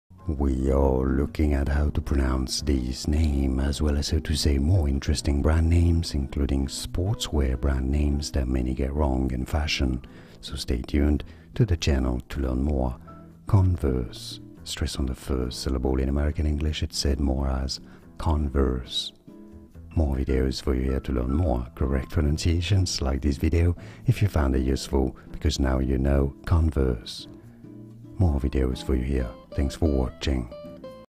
How to Pronounce Converse
This video shows you How to Pronounce Converse, pronunciation guide.
00:00 - Pronunciation Intro
00:08 - How to Pronounce Converse
00:20 - More Difficult Pronunciations